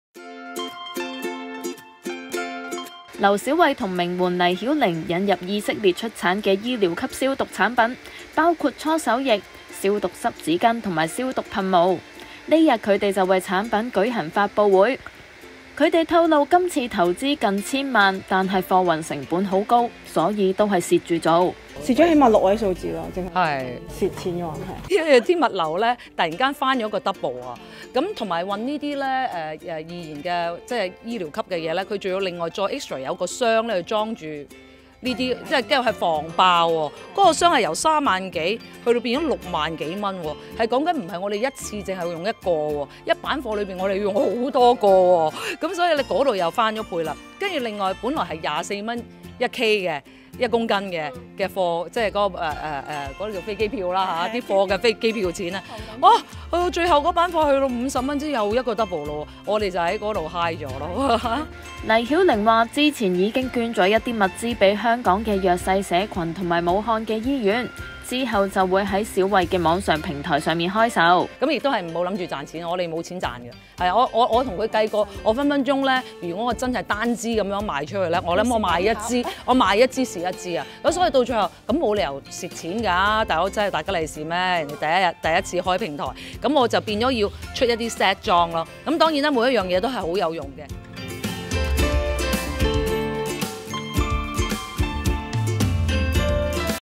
【娛樂】劉小慧引入消毒產品預咗蝕
劉小慧與名媛黎曉靈引入醫療級消毒產品，兩人舉行發布會。劉小慧表示：「因為成本高，運費好貴，所以預咗會蝕。」黎曉靈說，之前已捐了一些物資給香港弱勢社群，之後會於小慧的網上平台開售。

bmgproductionmusic